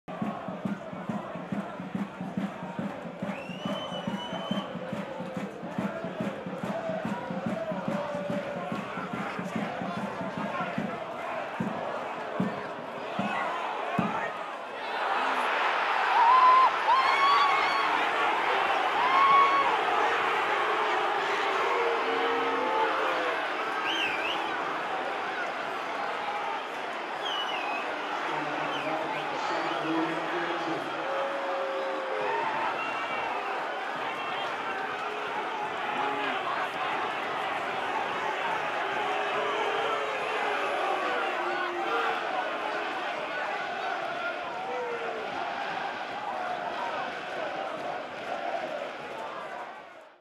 Rory McKenzie opens Ayrshire Derby scoring at Somerset
The Killie icon latched onto Fraser Murray's superb pass to score our first in a 3-1 win.